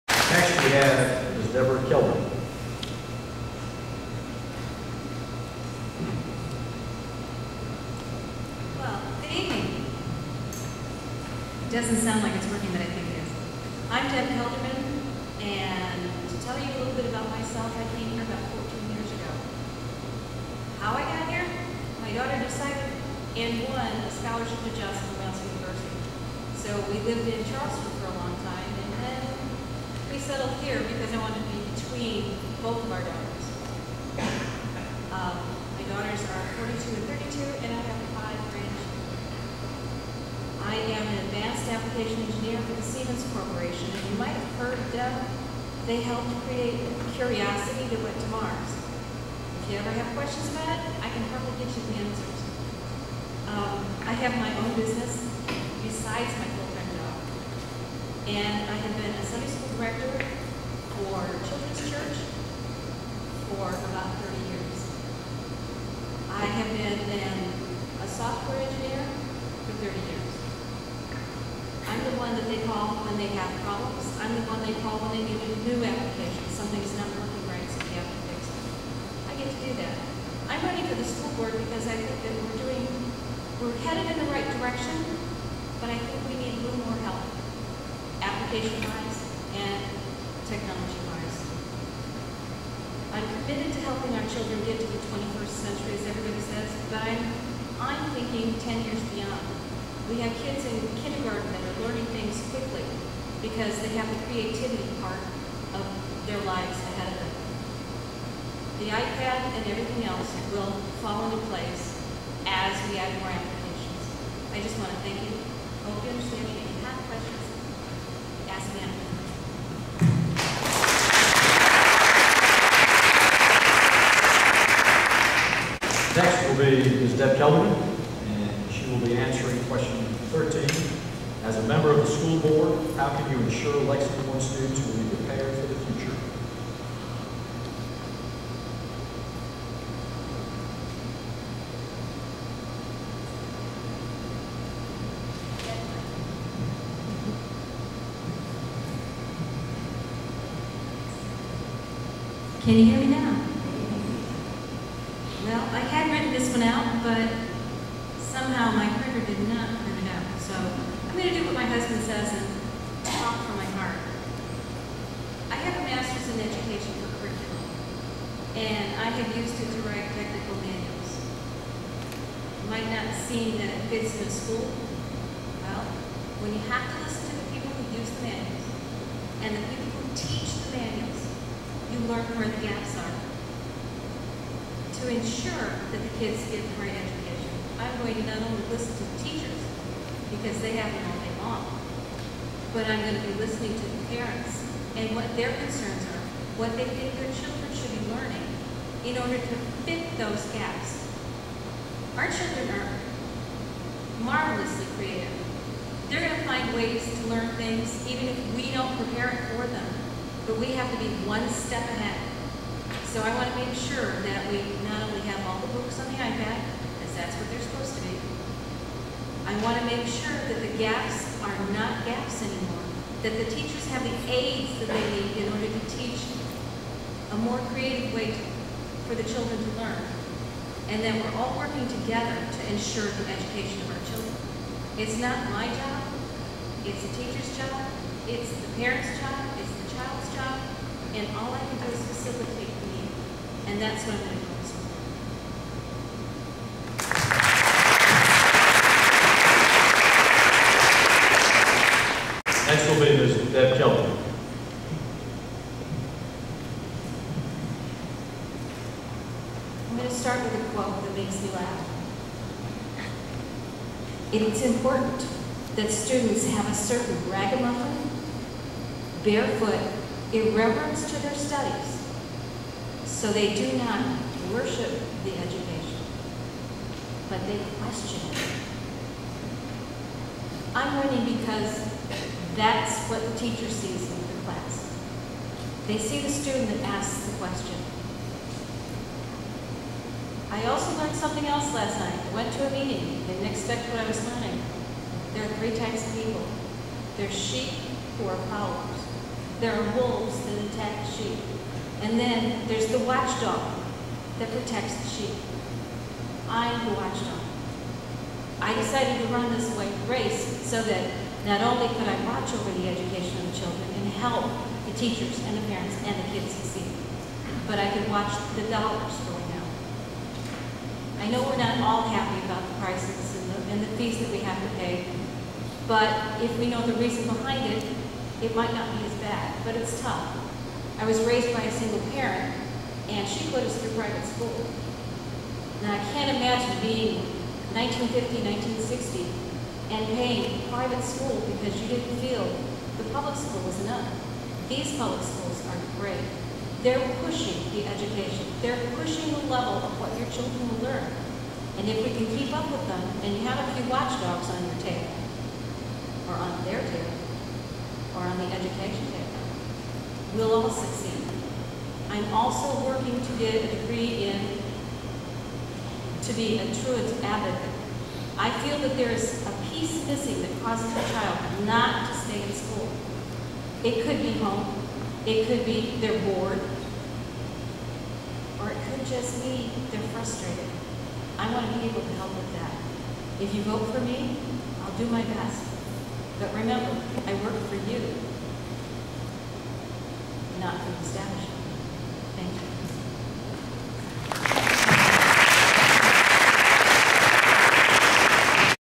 Deb Kelderman/ Candidate Forum #2 for Lex. School Board .mpg
This video is Deb Kelderman answering questions at Pleasant Hill Middle School Candidate Forum #2.